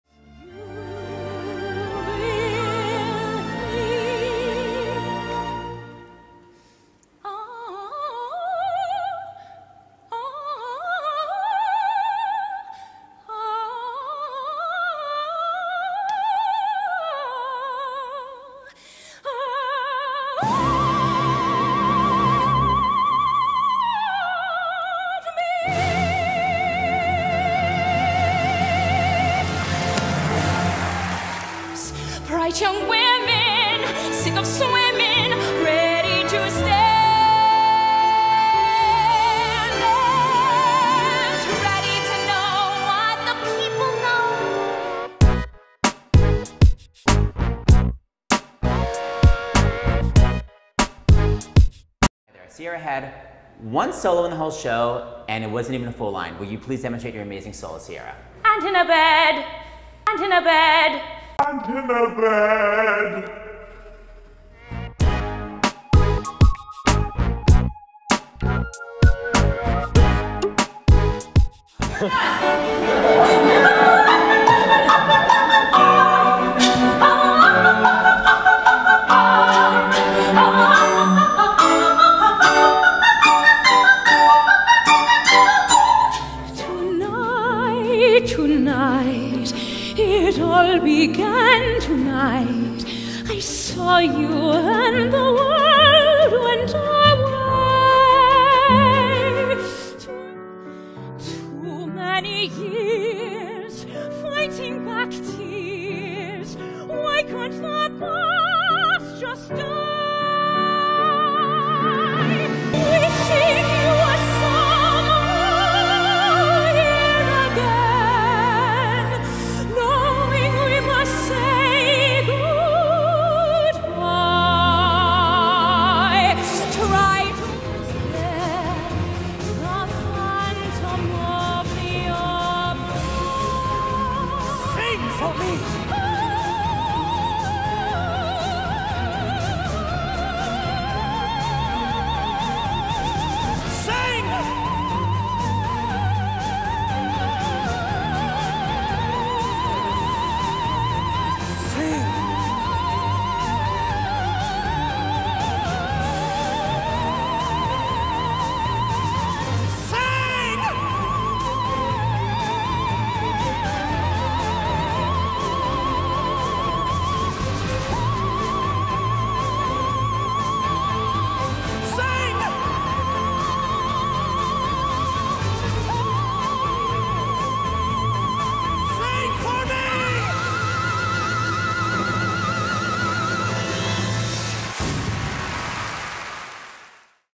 Sierra Boggess (Short Showreel)
For my BTEC Performing Arts Presentation.

UNIT TWO: Developing Skills and Techniques for a Live Performance.